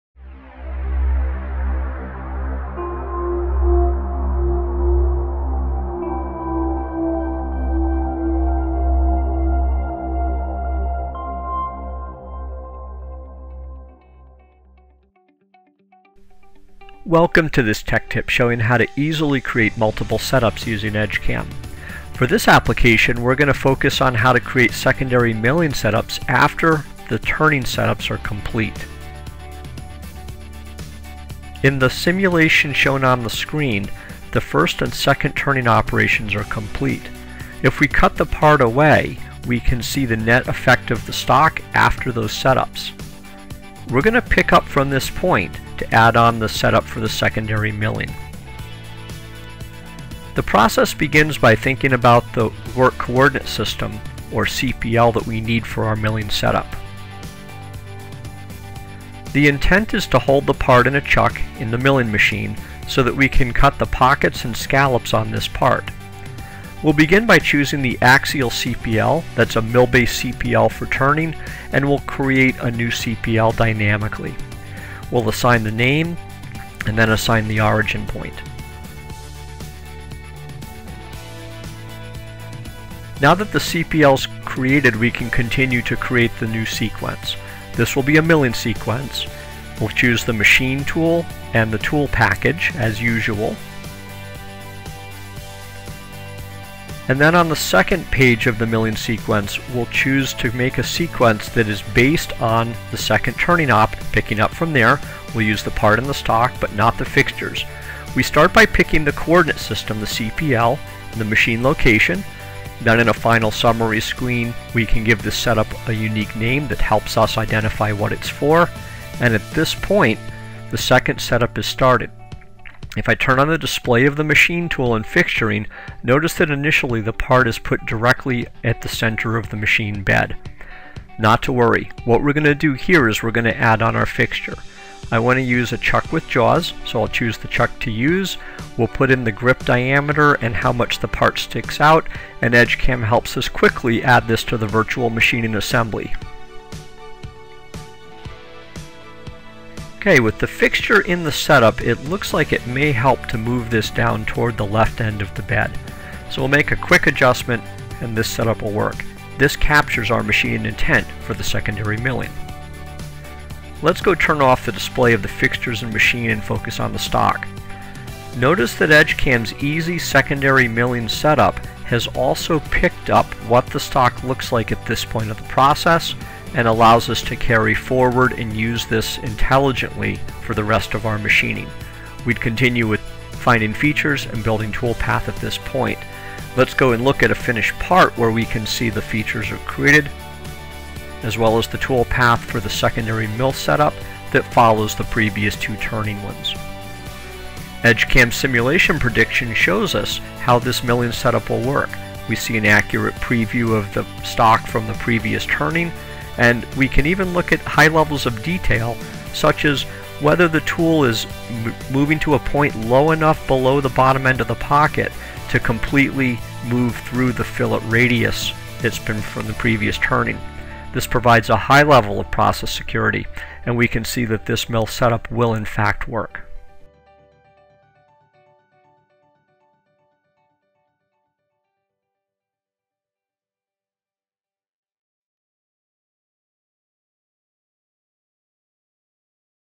Multiple Setups Milling after Turning
Watch as one of our application engineers demonstrates how to create a new sequence for a secondary milling setup, which will follow the previous turning operations!